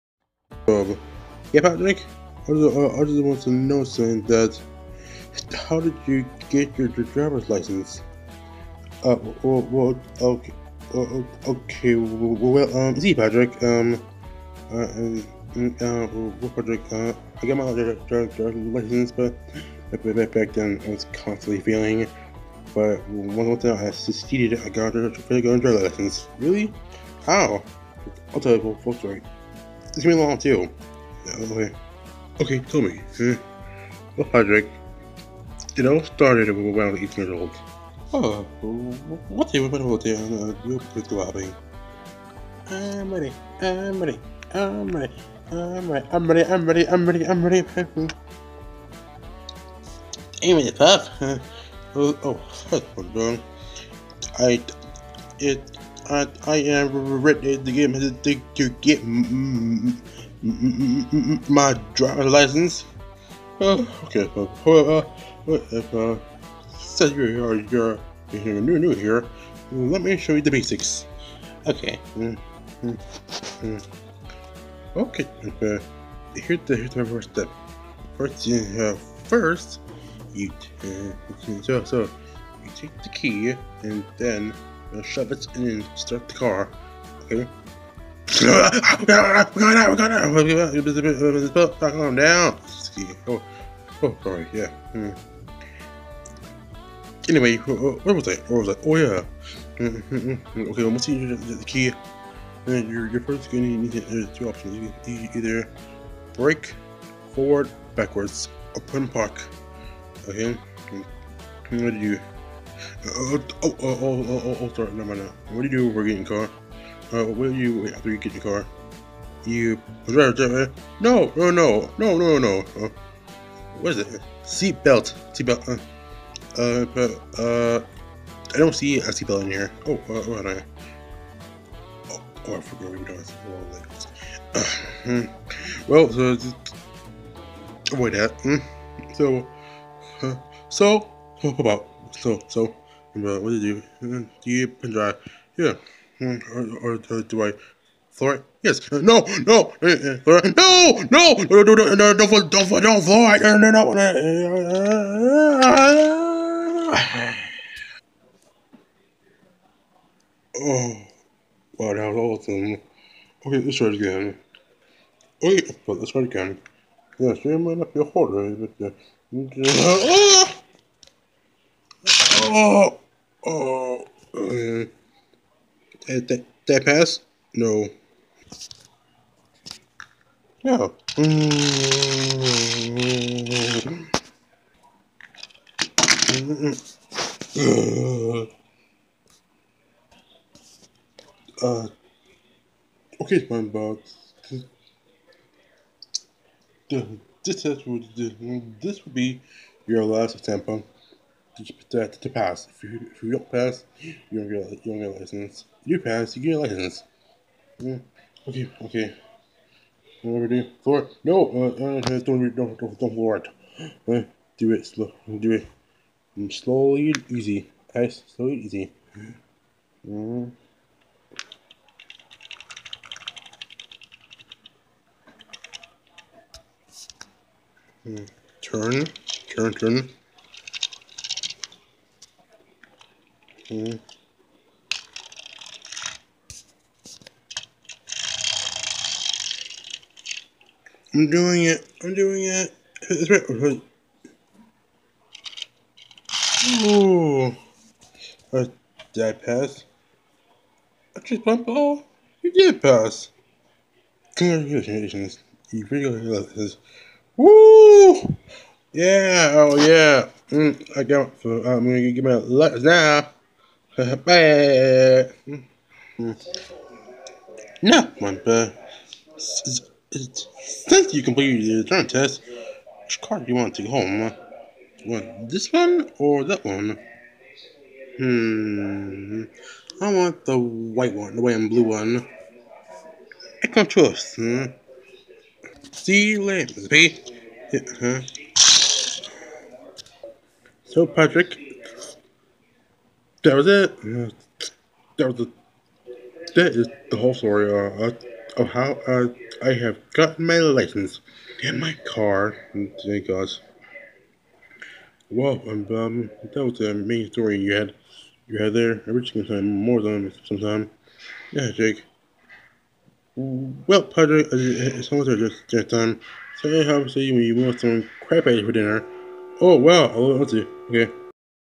Lego Spongebob: Boating School
Spongebob tells of how he got his drivers license